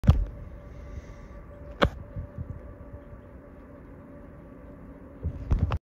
Vancouver Island Marten
This Marten was very curious about me in a tree stand.